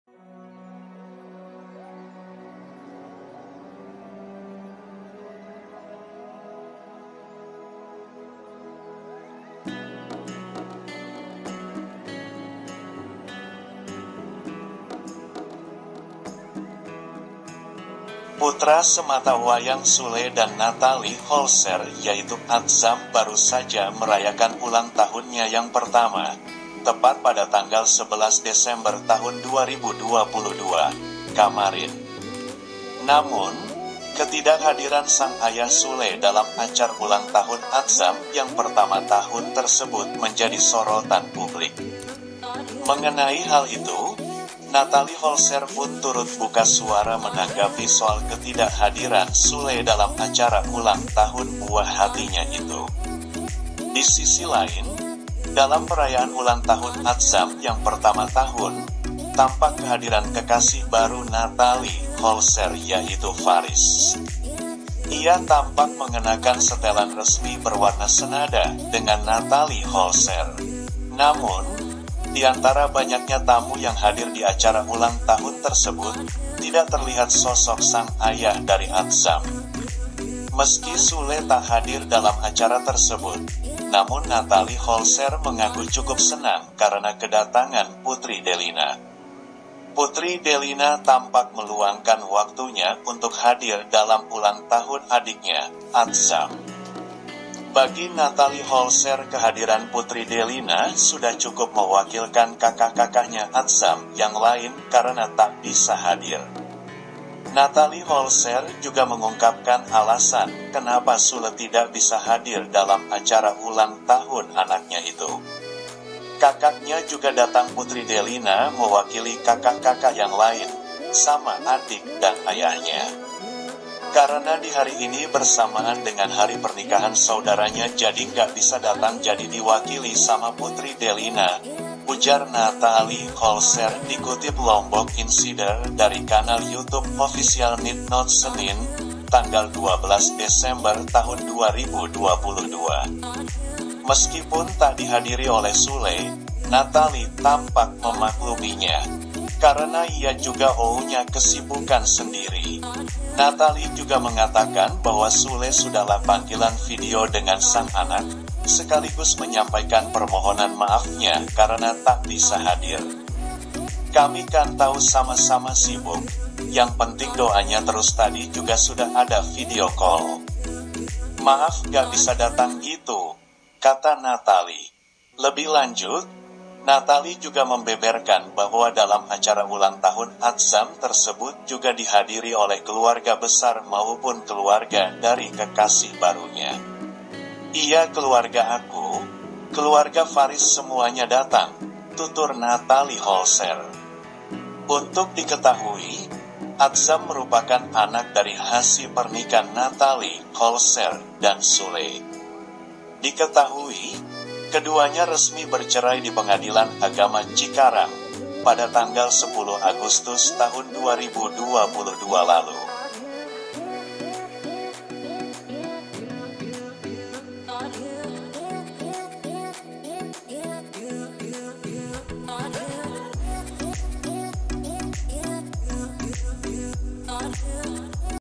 SULE TAK HADIR DIACARA ULANG TAHUN ADZAM, BEGINI TANGGAPAN NATHALIE HOLSCHE
Sule tak hadir diacara ulang tahun Adzam, begini tanggapan Nathalie Holscher